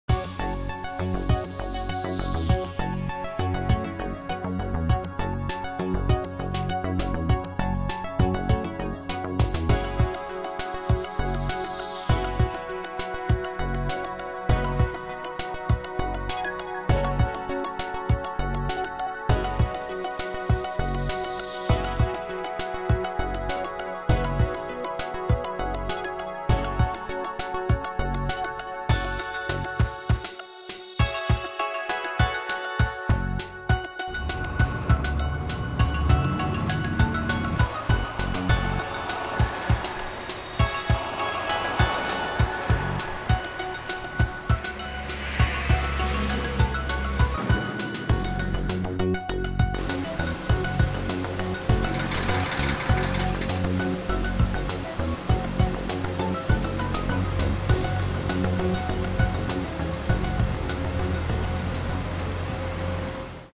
LEGO: 60020 Cargo Truck
LEGO set 60020 - Cargo Truck building and animation. LEGO City truck and forklift.